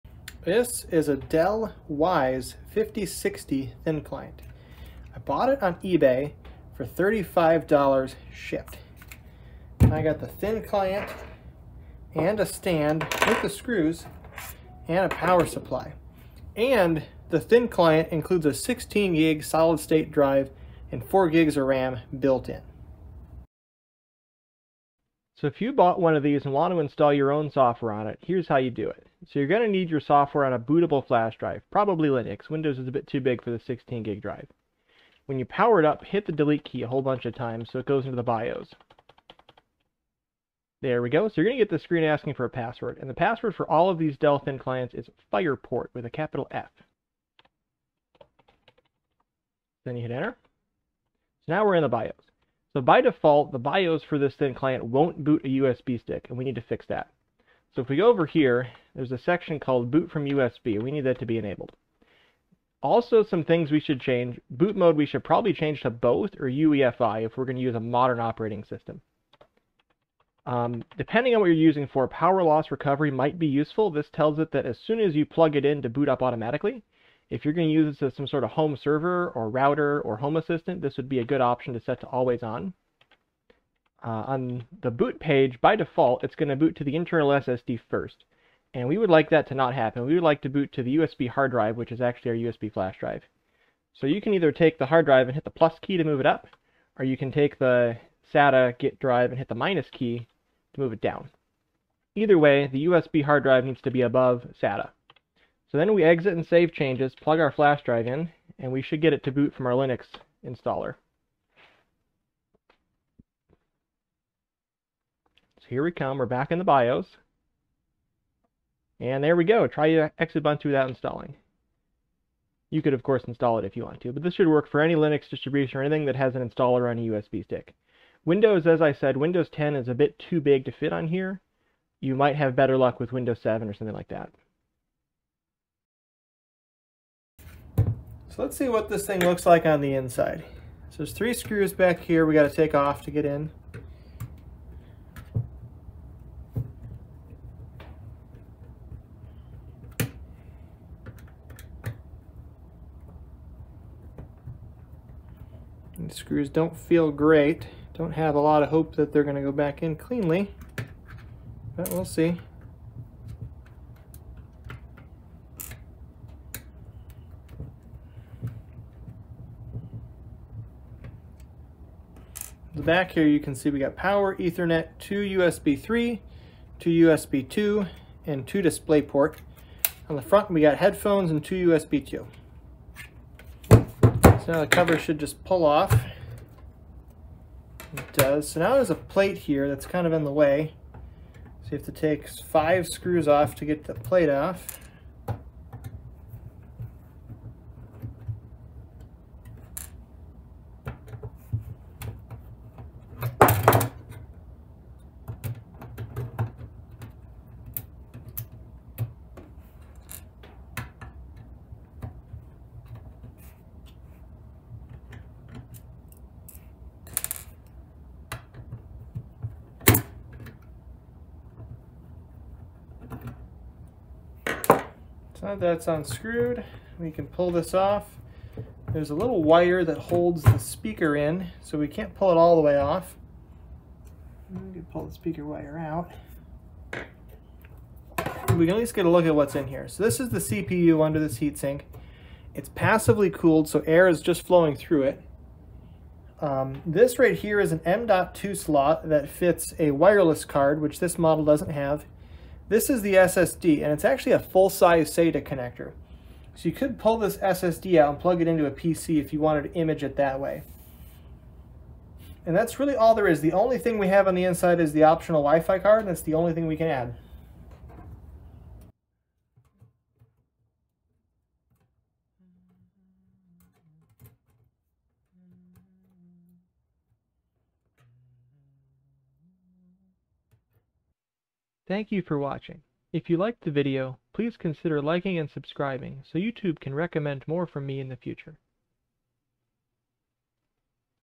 Dell Wyse 5060 Thin Client TEARDOWN & SOFTWARE INSTALL | Not Much Inside, but still great for Linux
Casually tearing down the Dell Wyse 5060 Thin Client that I bought for my home studio. Hopefully someone finds this useful, since these machines show up for sale on eBay all the time. It's a powerful box for the price, easily outperforming Raspberry Pi's in the same price class and including a power brick and storage too!

tl;dr the CPU supports AMD-V and AES-NI, the GPU supports H264 transcode, and it has a Realtek NIC

This Thin Client would make a great home server or Home Assistant host. Keep your eye out for deals, but make sure you know what you're buying (CPU, RAM, Storage, power supply included?)

If you find my content useful and would like to support me, feel free

Timestamps:
00:00 - Introduction
00:24 - Unlocking the BIOS
02:16 - Teardown